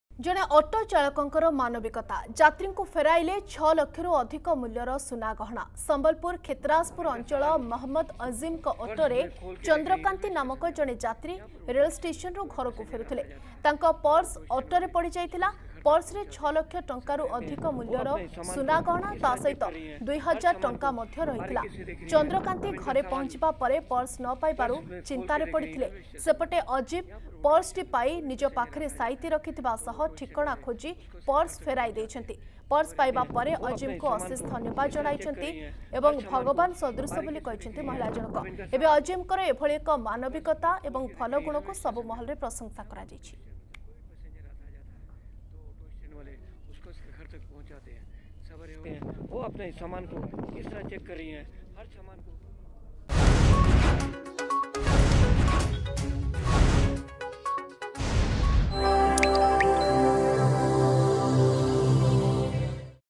ଅଟୋ ଚାଳକଙ୍କ ମାନବିକତ‍ା
ଅଟୋ ଚାଳକଙ୍କ  ମାନବିକତ‍ା । ଯାତ୍ରୀଙ୍କୁ ଫେରାଇଲେ ୬ ଲକ୍ଷରୁ ଅଧିକ ମୂଲ୍ୟର ସୁନା ଗହଣା । ସମ୍ବଲପୁର  କ୍ଷେତରାଜପୁର ଅଂଚଳ ମହମଦ ଅଜିମଙ୍କ ଅଟୋରେ ଚନ୍ଦ୍ରକାନ୍ତି ନାମକ ଜଣେ ଯାତ୍ରୀ ରେଲଷ୍ଟେସନରୁ ଘରକୁ ଫେରୁଥିଲେ ।